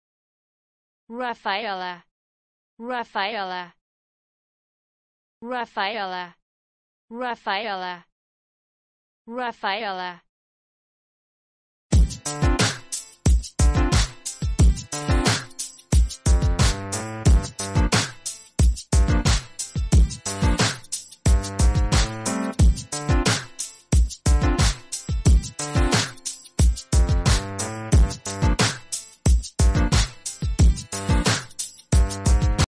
How do you say 'Raphaela' in Spanish?
How to say 'Raphaela' in Spanish? Listen to hear the pronunciation.